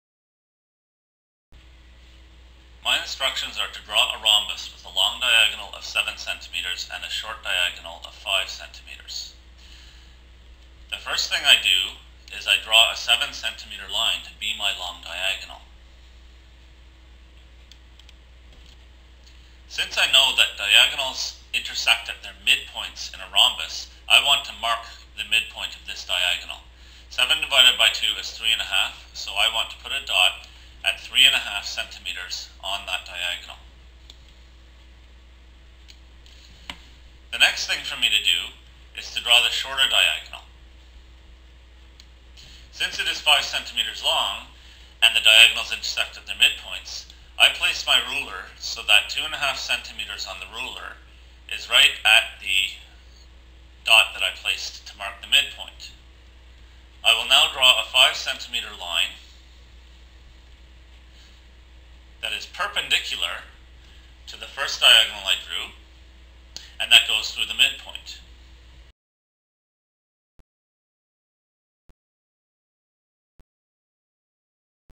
Draw a Rhombus
This video illustrates how to draw a rhombus, as is required knowledge in basic high school Geometry courses. For more instructional videos, as well as exercise and answer sheets, go to: